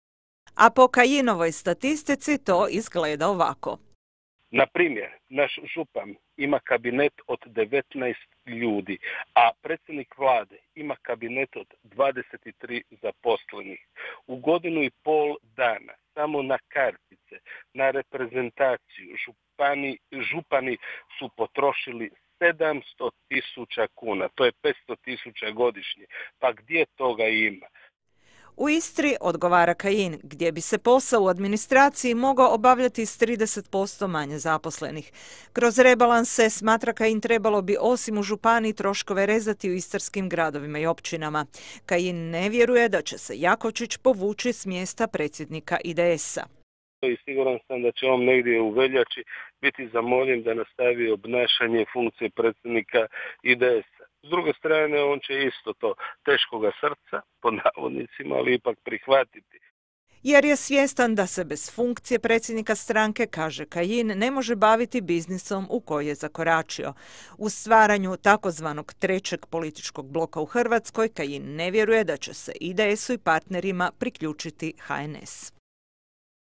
Kajin za rezove u Županiji
Neoprostiva je rastrošnost Županijske vlasti u Istri, uvjeren je saborski zastupnik Damir Kajin. Upozorava na previše zaposlenih u županijskim službama i velike troškove reprezentacije.